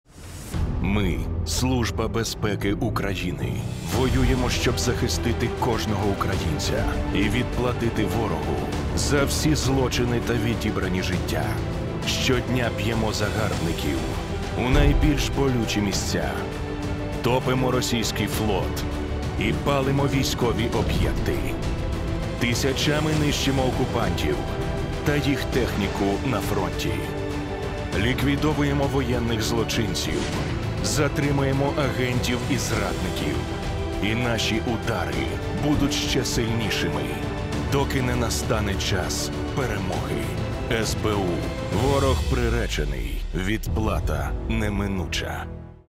Ворог приречений – відплата неминуча: СБУ відзначає професійне свято
Сьогодні, 25 березня, День Служби безпеки України.
Редакція «ЧЕЛАЙН» вітає співробітників СБУ в Чернігівській області з професійним святом.
Дякуємо за співпрацю і боротьбу.
Слава Україні.